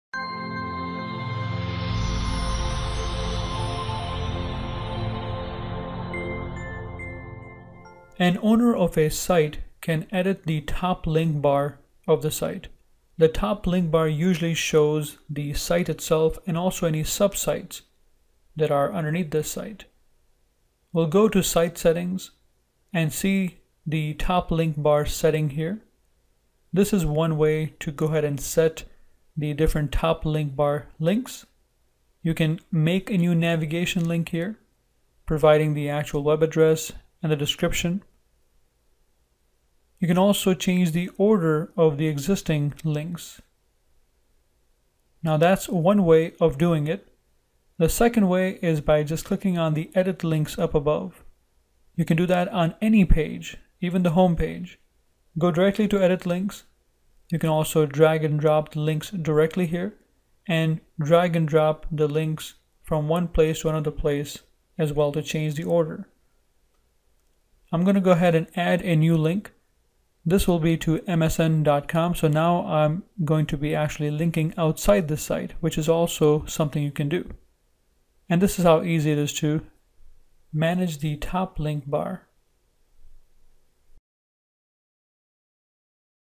How to Configure the Top Link Bar in SharePoint 2016
This short video explains How to Configure the Top Link Bar in SharePoint 2016.
Watch how you can extensively customize the top link bar to meet the needs of your SharePoint Site users.